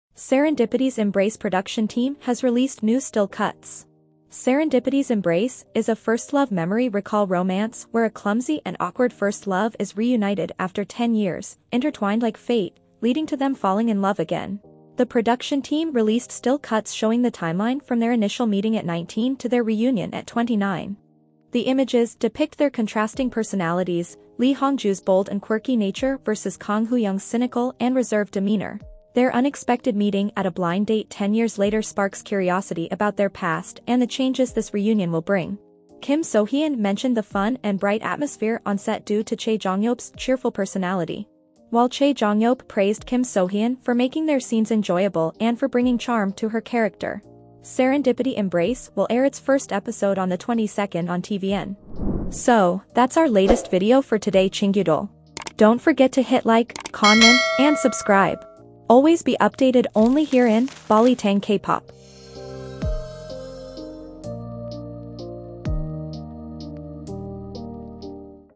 Serendipity’s Embrace | FIRST LOOK STILL CUTS | Kim So Hyun | Chae Jong Hyeop
tvN’s upcoming drama “Serendipity’s Embrace” (previously known as “Is It Fate?”) has shared a new glimpse of Chae Jong Hyeop in his starring role!